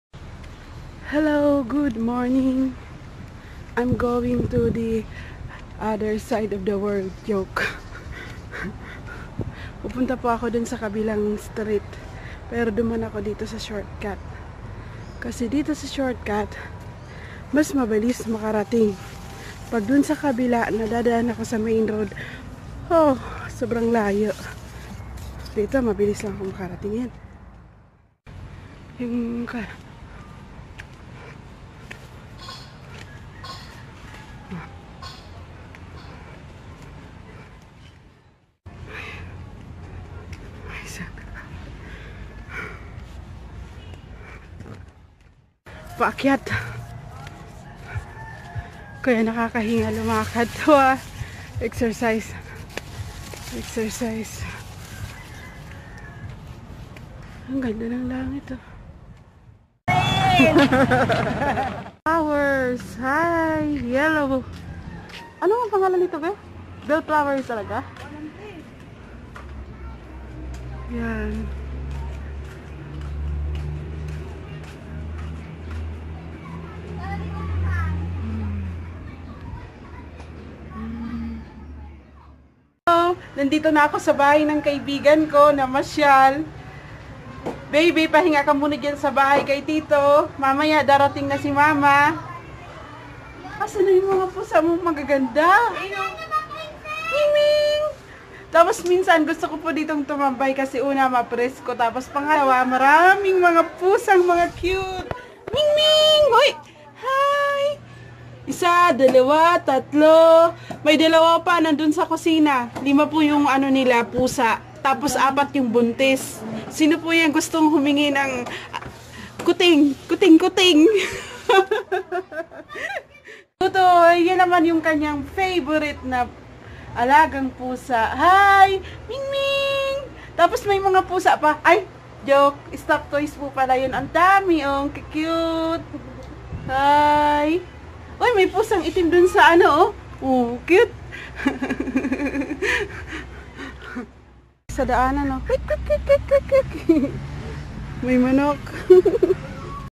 Short cut way mabilis dumaan,Humble lang kahit wala nasingil( tawa tawa )
Sa short cut mabilis dumaan,isang iglap lng sa kanto ka na kaibahan sa tamang daanan malayo nga safe nman parang bundok mapresko daanan kc po maniningil ako sa paupahan namin tatlo buwan din halos libre na april may dahil pandemya hai sana matapos na pandemya para normal na makapanghanapbuhay slamat po